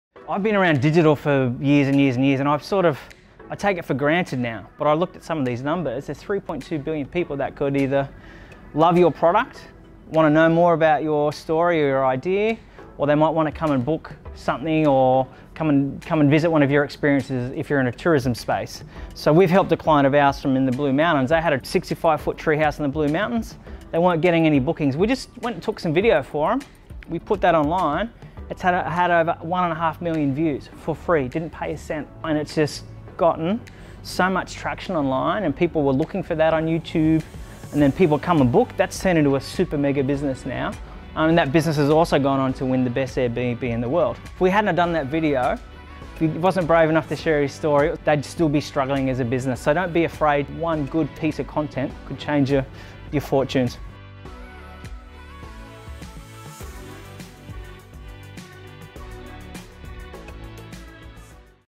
03 / 10 Getting Started With Video Content - one piece of content can change your life.
Director of award nominated creative content agency recently spoke with a cohort of Indigenous business leaders about how they can get start with video marketing to share their unique product, services & business USP.

to bring the workshop to life & have the support of the City of Sydney & Investible.

The interactive 1x hour session covered the below topics:
- Why make content?
- Mobile production basics.
- Short form video basics.
- A DIY practical session & interactive Q&A.

The session covered off currently global internet usage rates (approx. 4.5 billion), the importance of making mobile first content (short snackable pieces), composition basics incl. the rule of thirds, through to basic film / lighting & publishing options.

Key platforms like LinkedIn, YouTube, Facebook & Instagram were covered with tips on how to get discovered (hashtags, key words & tagging) through to which platform might be best to reach the right audience & drafting a script outline.

'I hope you enjoy these video snippets, the hardest step is usually the first step & over coming your nerves, the more you do it, the easier it get's! Happy creating.' Jamie A.

Feel free to get in touch should you wish to have Jamie speak, present or develop training materials for your team.